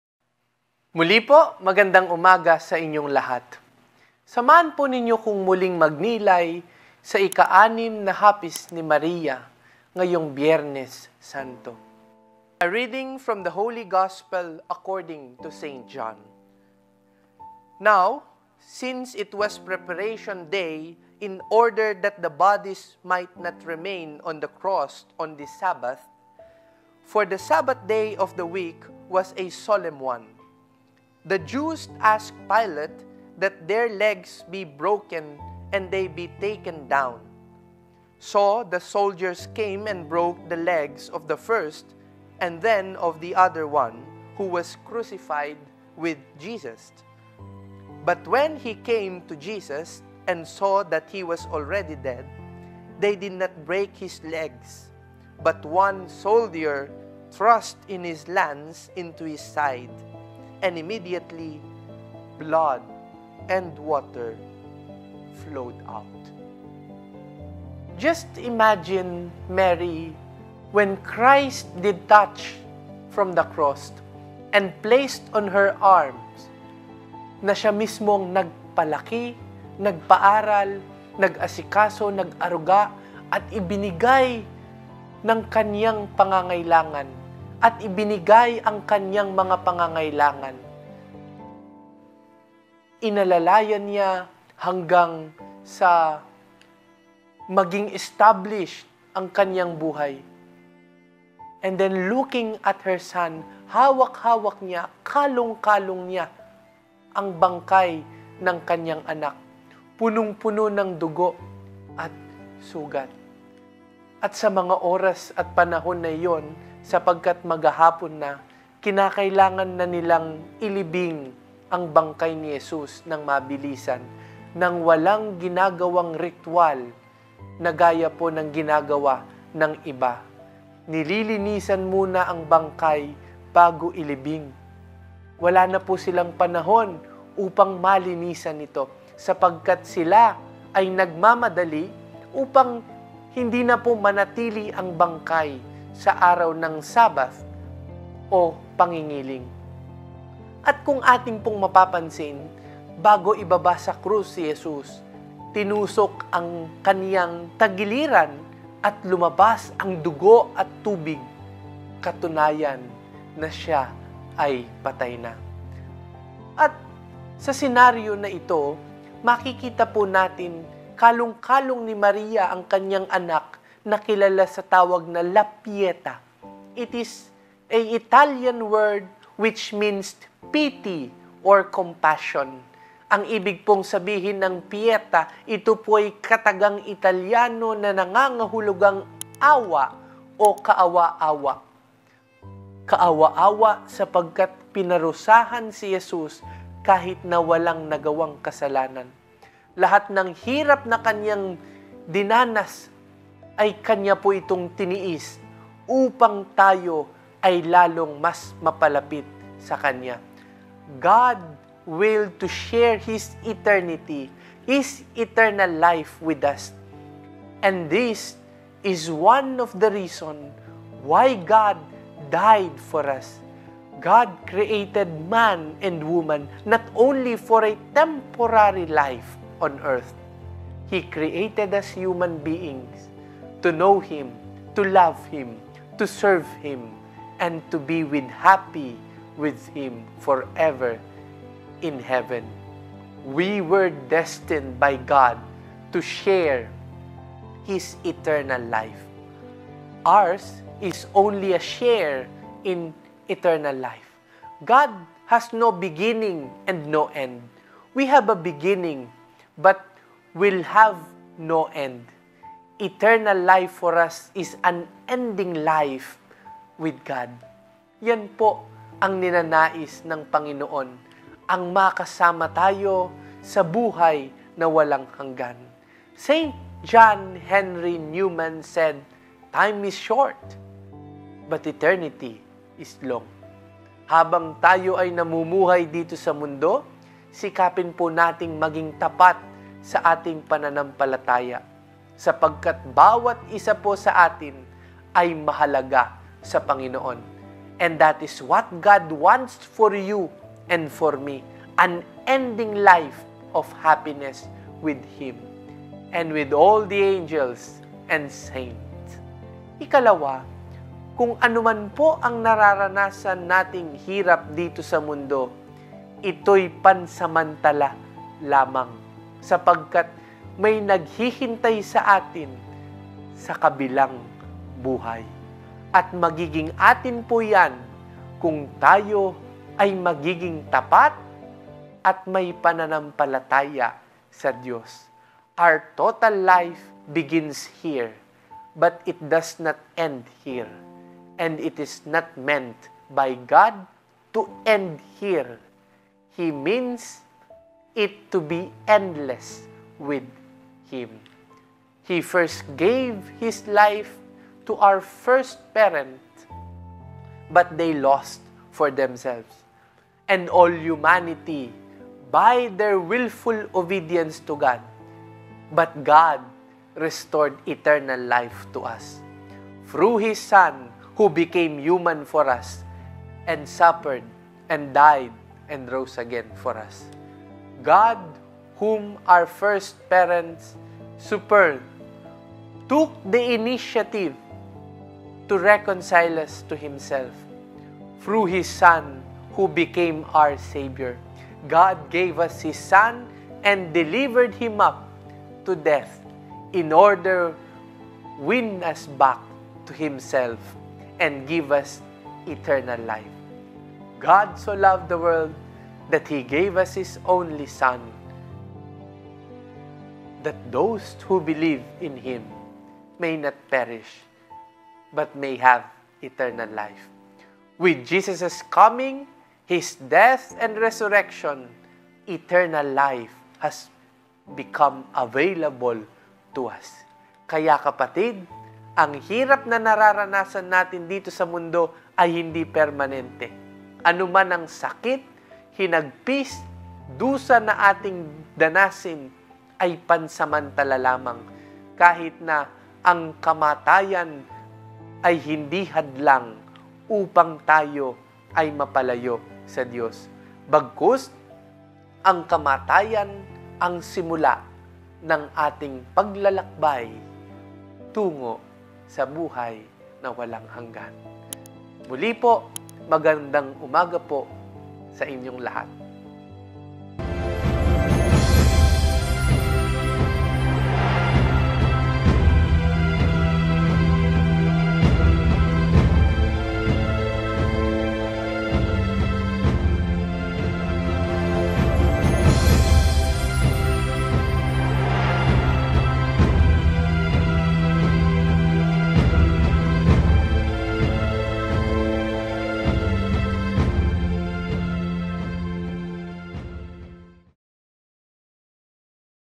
The Sixth Sorrow of Blessed Virgin Mary | The Retrieval of Jesus' Body
Seminarian Paul Adrian Layug  conducts a Seven-Day Reflections on the Seven Sorrows of the Blessed Virgin Mary here @ ETVN Philippines this Holy Week. Starting Palm Sunday (March 24, 2024) to Black Saturday (March 30, 2024).